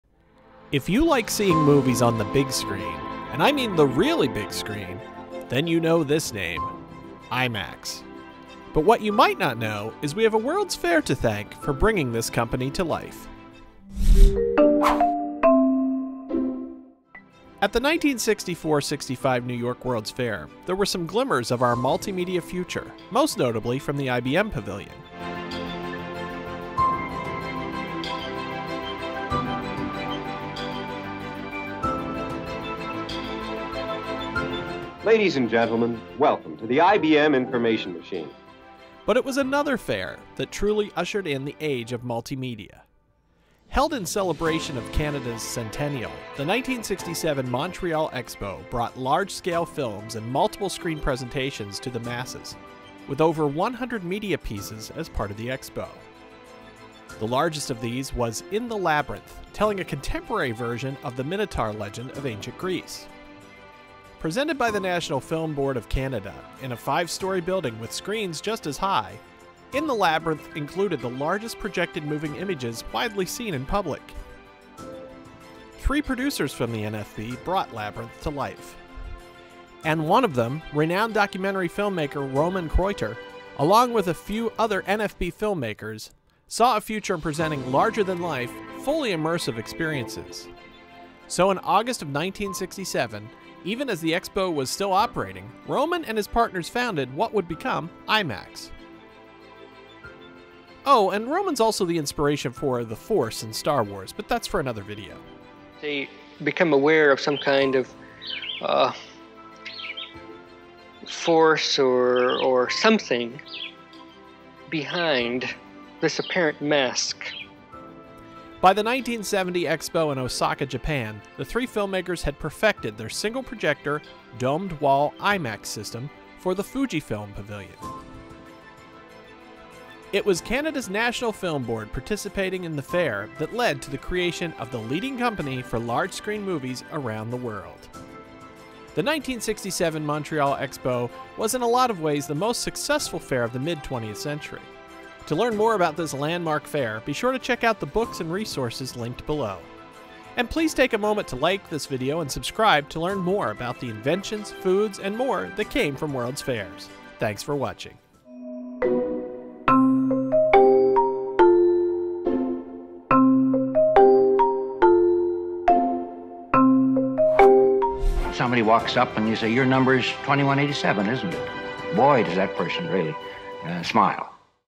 It Came From The Fair: The little-known story of how IMAX began thanks to the 1967 Montreal expo.
Not only TV (and color TV) debuted at world's fairs, but we also have a world's fair to thank for bringing big-screen movie viewing to the masses. We're taking a look at the founding of IMAX, and how it all ties back to the 1967 Montreal Expo, otherwise known as Expo '67.